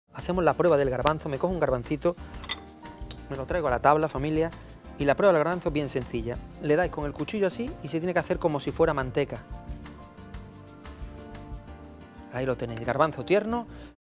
Cómo averiguar si el garbanzo está bien cocinado
Conseguir que las legumbres estén cremosas y en su punto tiene su ciencia. Siempre la experiencia nos dirá cual es la mejor forma de conseguirlo pero no está de más seguir unos buenos consejos para alcanzarlo. Enrique Sánchez nos da la clave para averiguarlo, como siempre, de la experiencia sale el mejor consejo.